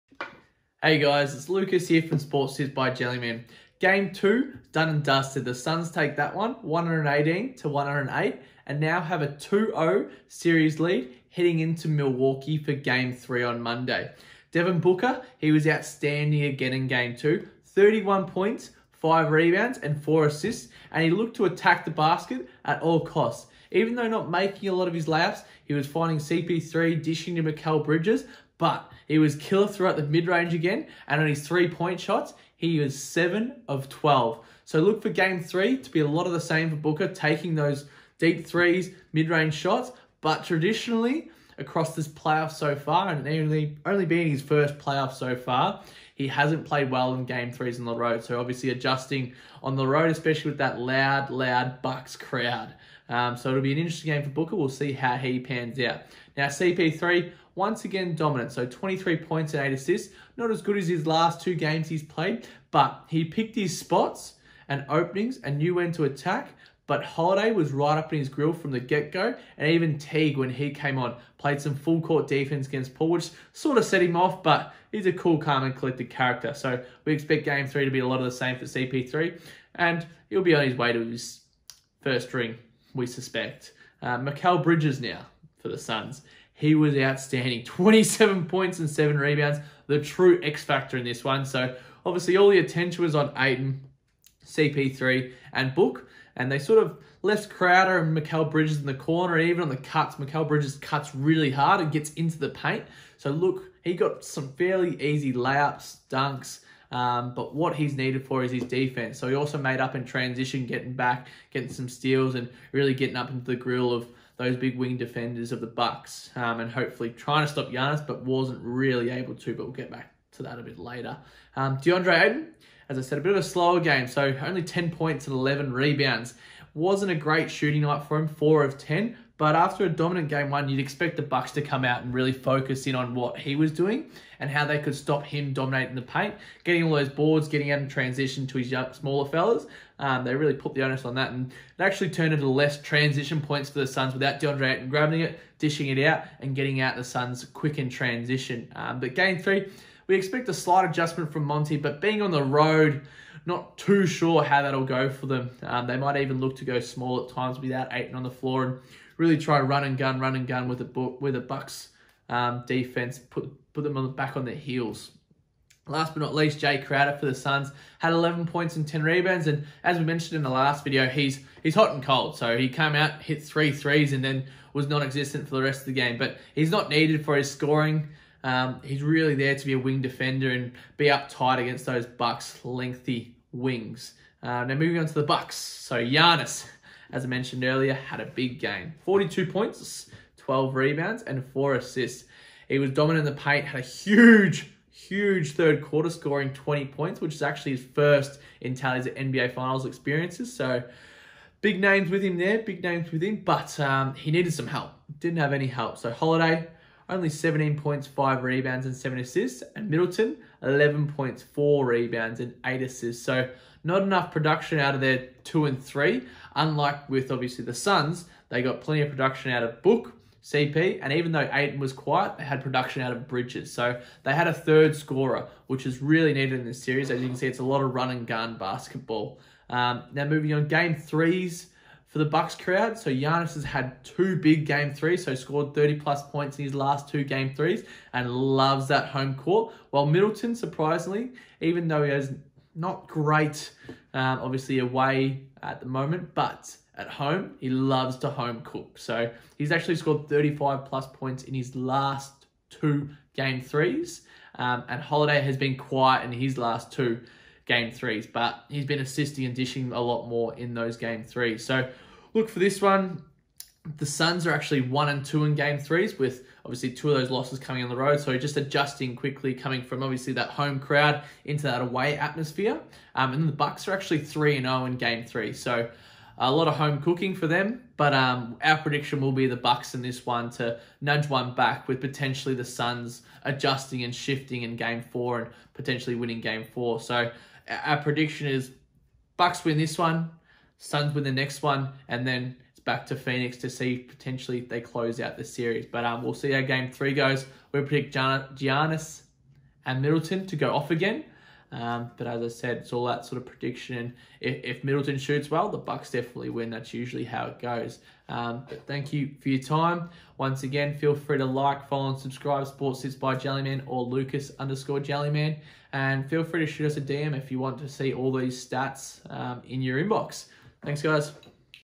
NBA Finals: Bucks vs Suns (Game 02 Review & Game 03 Preview)
Lucas Jellyman-Turner From Sports Tips By Jellyman gives us an overview & preview of NBA Finals Games 02 & 03 between the Milwaukee Bucks & the Phoenix Suns.

 - Suns won Game 02: 118-108
 - Suns go up 2-0 in the series with Game 03 now in Milwaukee.

 - D.Booker 31 Points, 5 Rebounds & 6 Assists
 - C.Paul 23 Points & 8 Assists.
 - M.Bridges 27 Points & 7 Rebounds.
 - D.Ayton 10 Points &  11 Rebounds.
 - J.Crowder 11 Points & 10 Rebounds.

 - G.Antetokounmpo 42 Points, 12 Rebounds & 4 Assists.
 - J.Holiday 17 Points, 5 Rebounds & 7 Assists
 - K.Middleton 11 Points, 6 Rebounds & 8 Assists.
 - P.Connaughton chipped in with 14 Points & 7 Rebounds.

 - Game 03’s have been big for Giannis scoring 30+ points in his last two.
 - Middleton feels more comfortable at home as well scoring 35+ points in his last two Game 03’s

 - Suns 1-2 in Game 03’s
 - Bucks 3-0 in Game 03’s

 - Our prediction is the Bucks to shoot the lights out at home Giannis & Middleton big games.